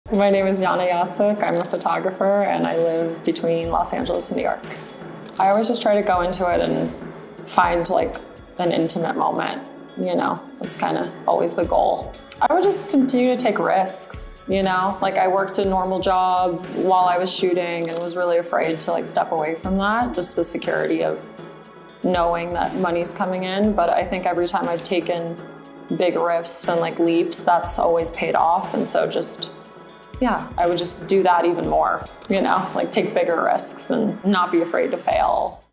Photographer Yana Yatsuk
Yana Yatsuk is in search of the authentic moment all the while embracing the variable of chance.